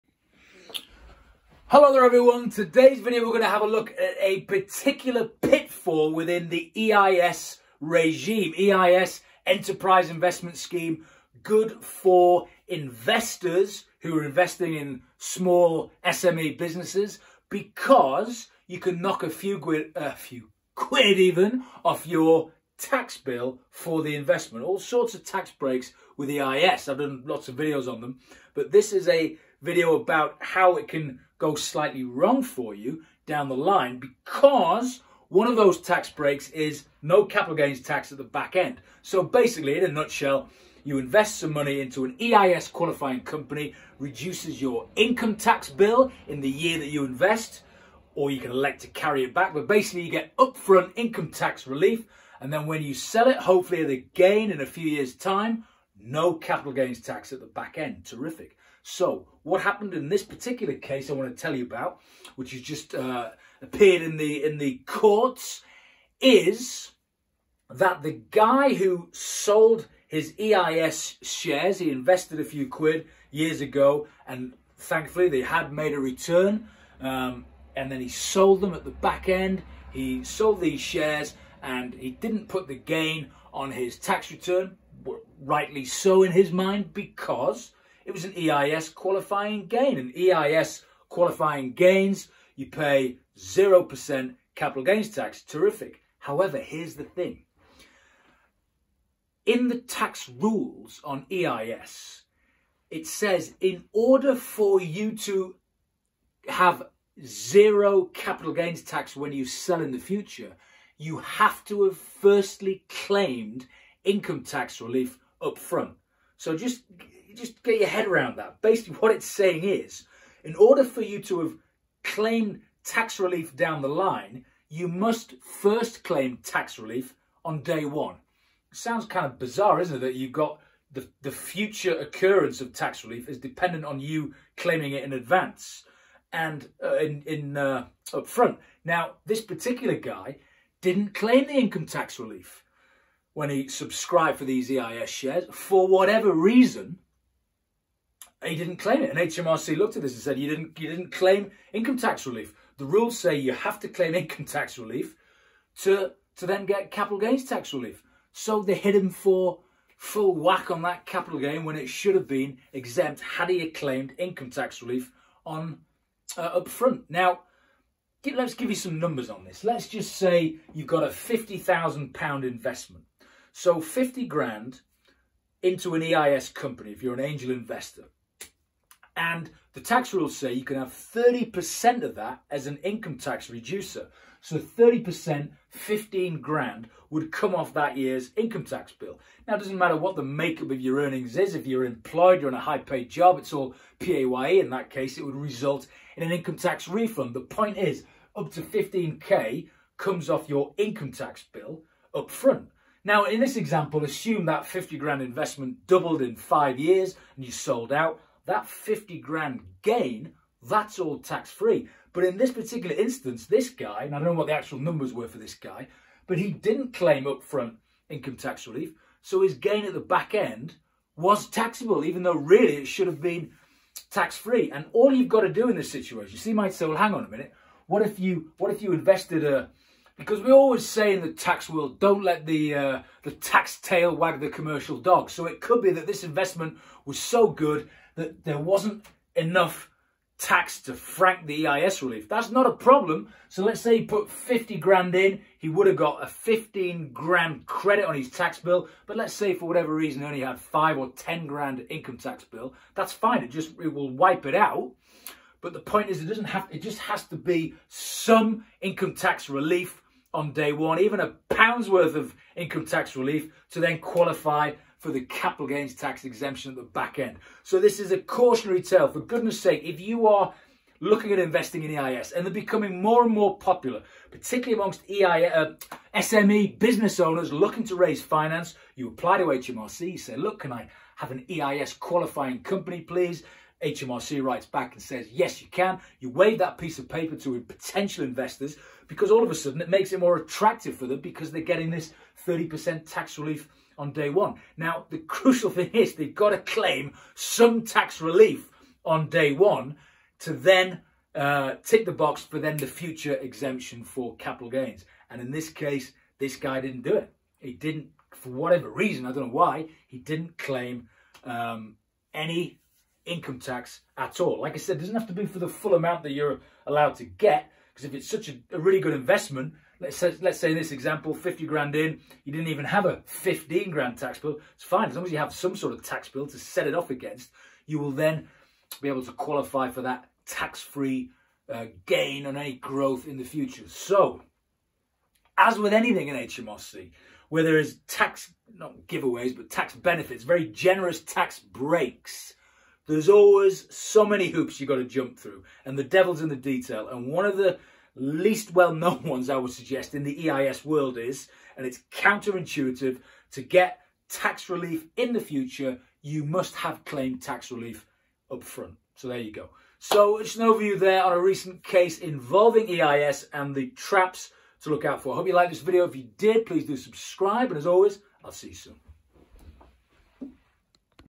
EIS Trap
In this video we take a look at a surprising tax trap embedded in the EIS legislation